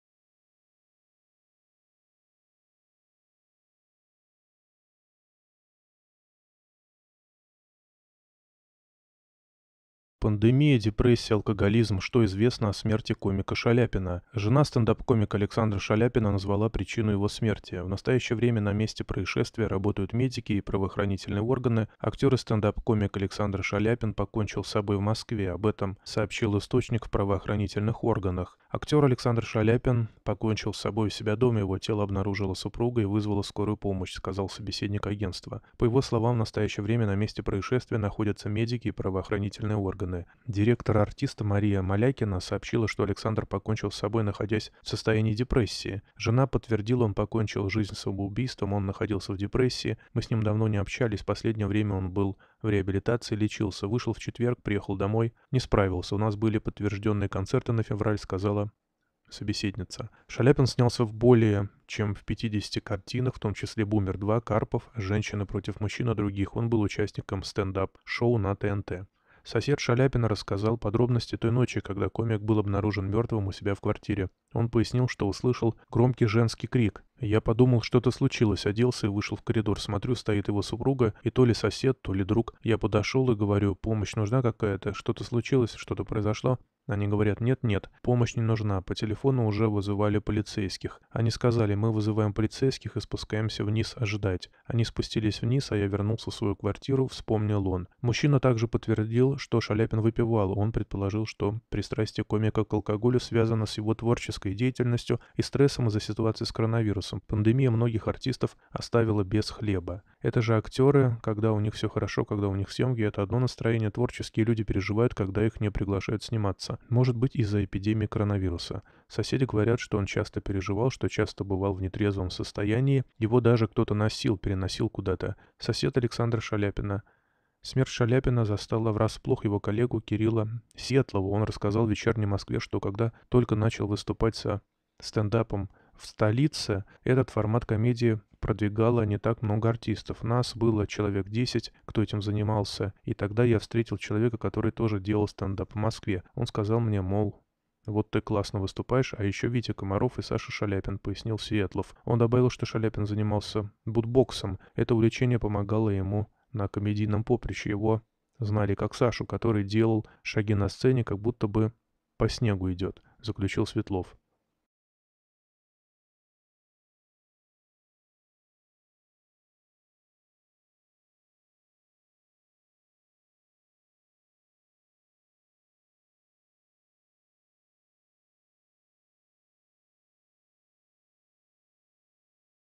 Ушел актер Александр Шаляпин. Ему был 41 год
В Москве ушел актер Александр Шаляпин. Ему был 41 год.